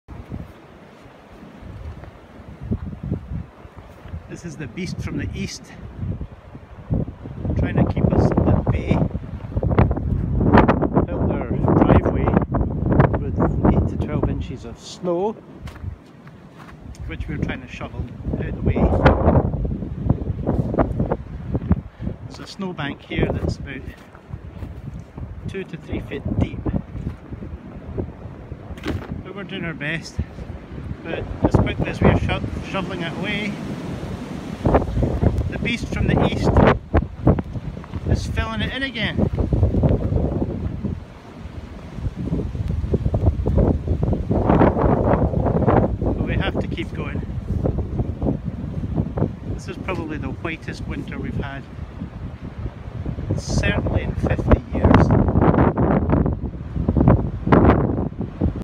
The Beast From The East
Fighting against nature, to try and clear our driveway of snow.